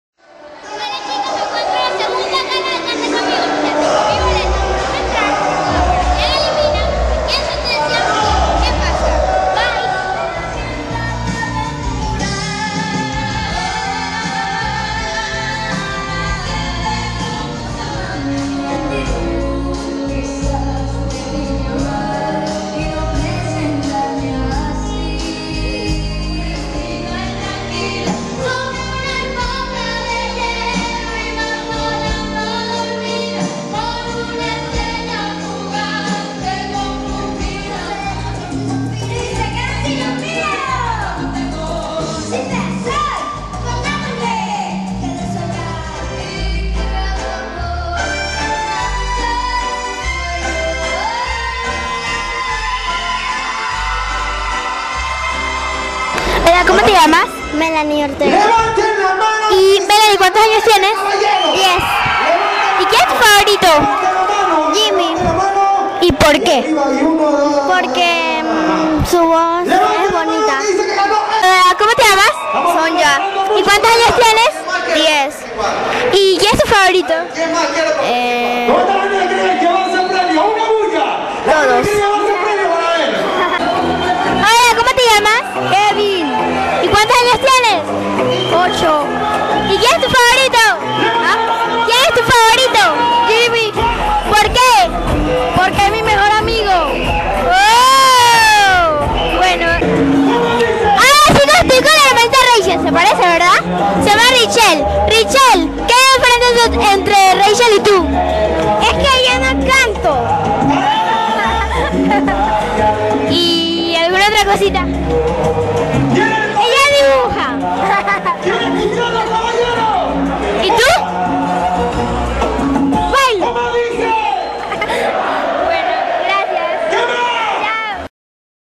ISA LA REPORTERA EN LA SEGUNDA GALA DE CANTA CONMIGO
¡Hola chicos! Soy Isa, reportera de Aprendo en La Prensa. Fui a la segunda gala de Canta Conmigo 2013 y, para los que no lo pudieron verla, les cuento que el tema era telenovelas. También entrevisté a la hermana de Rashell y a algunos niños del público. ¡Mira las entrevistas!